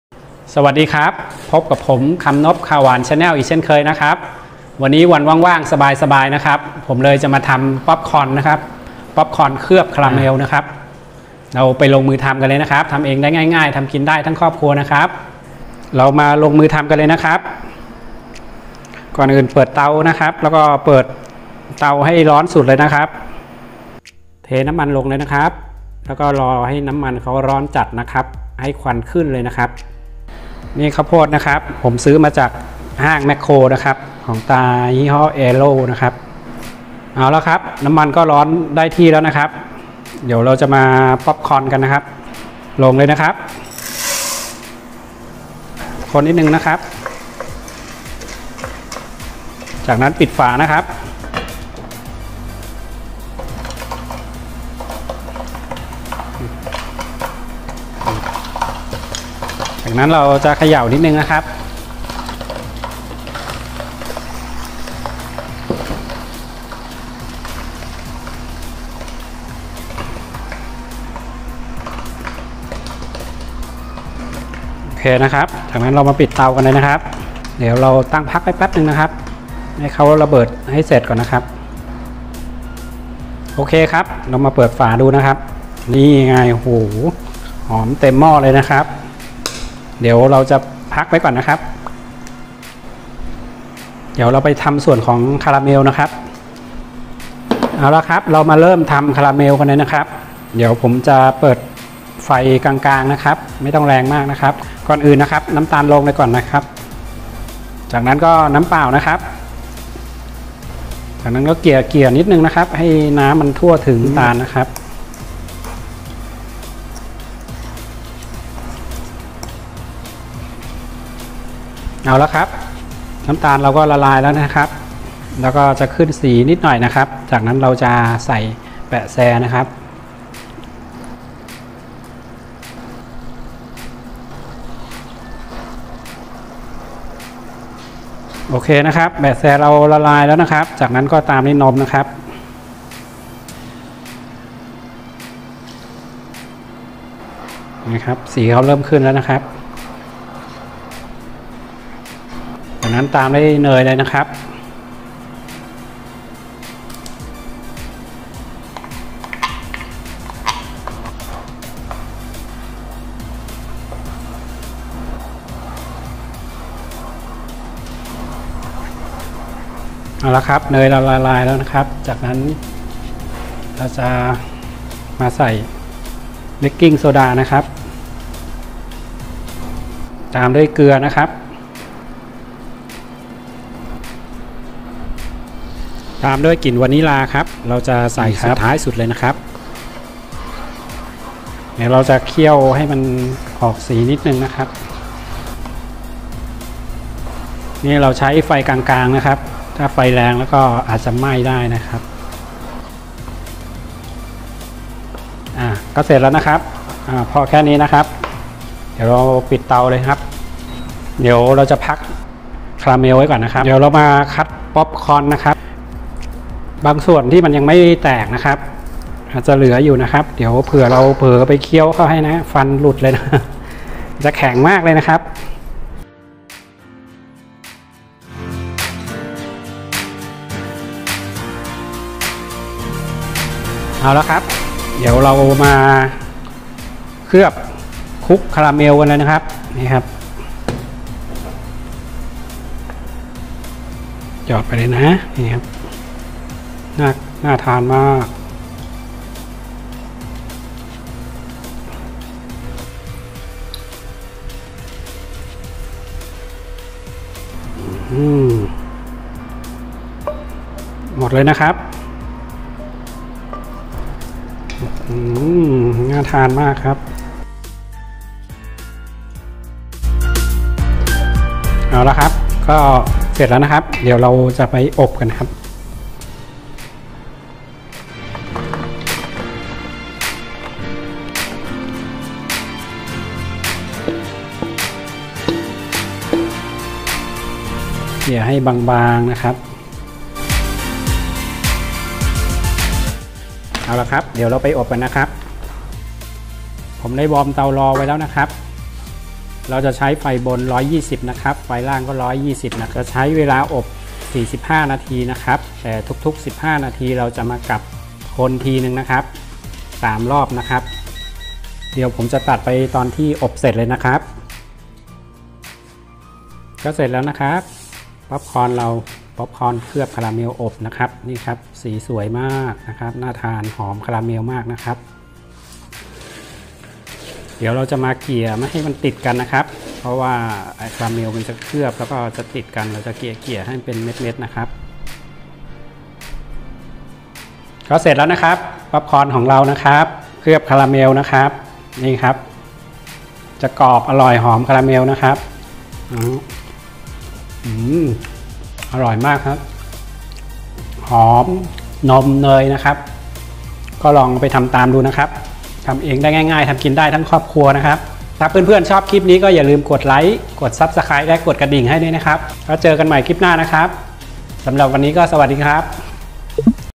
ป๊อปคอร์นเคลือบคาราเมล ทำเองได้ง่ายกรอบอร่อยทุกเม็ด
ป๊อปคอร์นคาราเมล (ส่วนผสม)

1.เม็ดข้าวโพด 1ถ้วย

2.น้ำมันพืช 1 ถ้วย

3.น้ำตาลทราย 320 กรัม

4.น้ำเป่ลา 80 กรัม

5.แบะแช 40 กรัม

6.นมสด 100 กรัม

7.เนยสด 100 กรัม

8.เบคกิ้งโซดา 1 ช้อนชา

9.เกลือ 1/2 ช้อนชา

10.กลิ่นวานิลา 2ช้อนชา